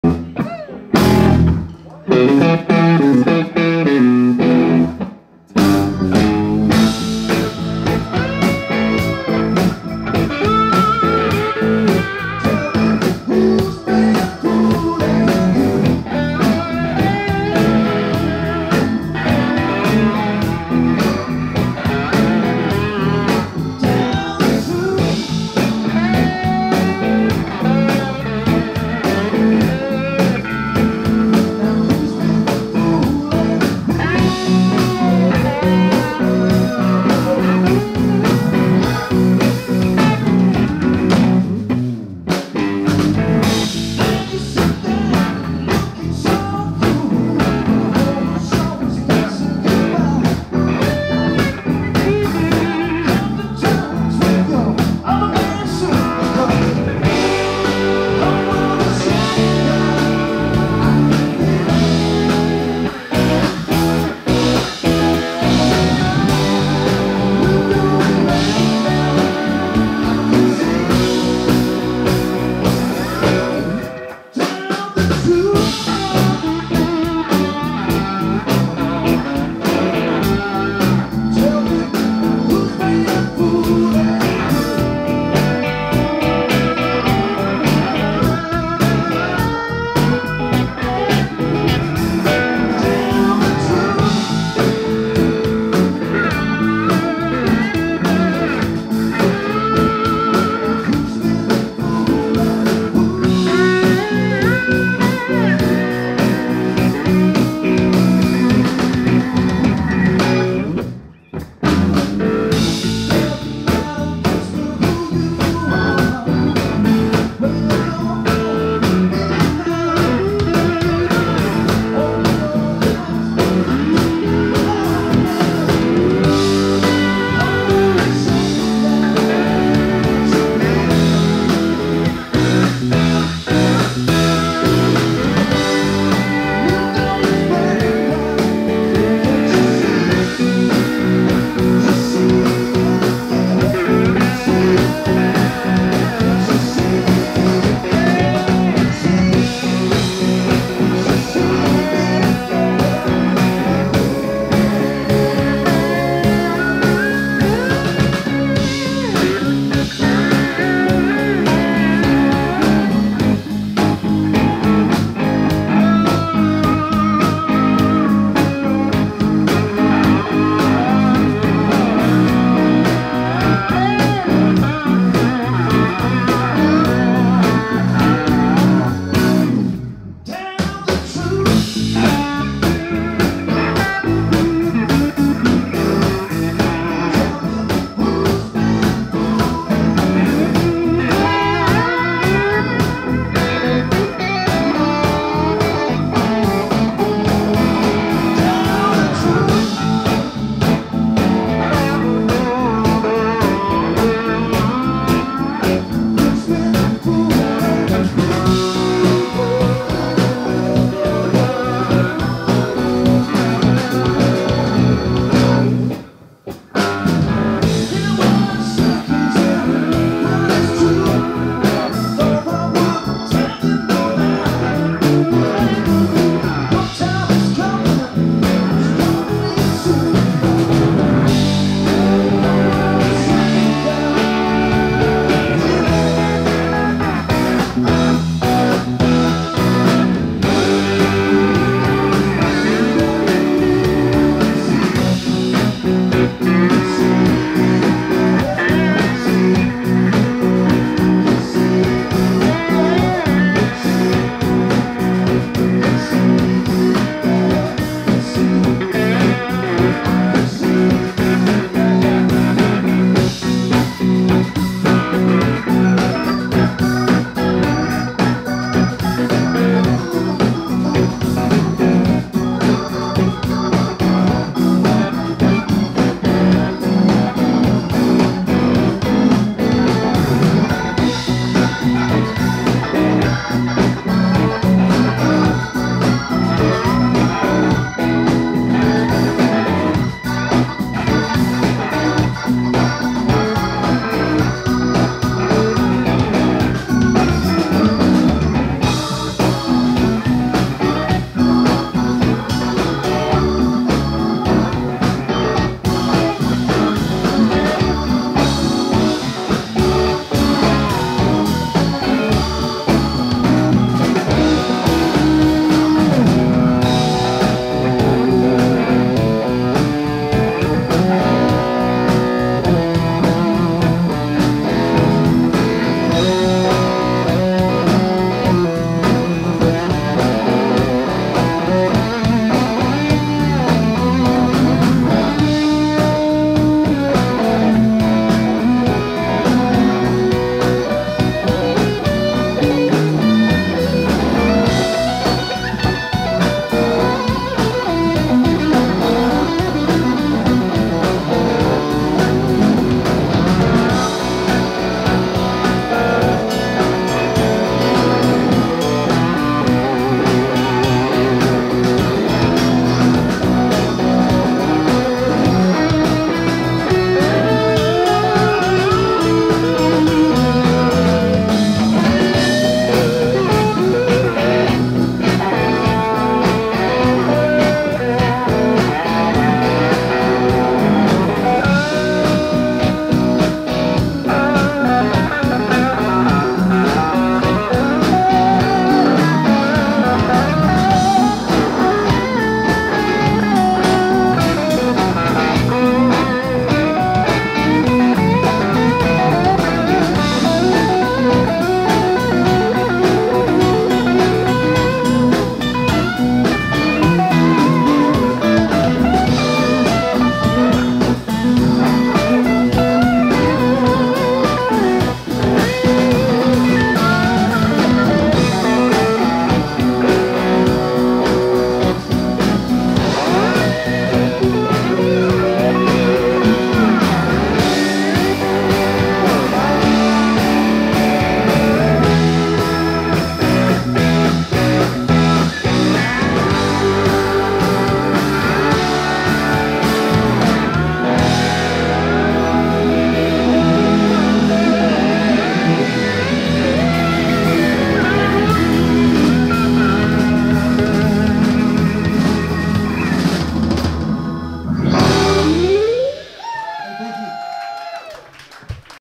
Jonathan Sloane Trio, wsgs - Bobby Thompson, Brian Simms, 8x10, 2-24-2022
Check out this epic venue in Federal Hill - Baltimore, MD:
The 8x10: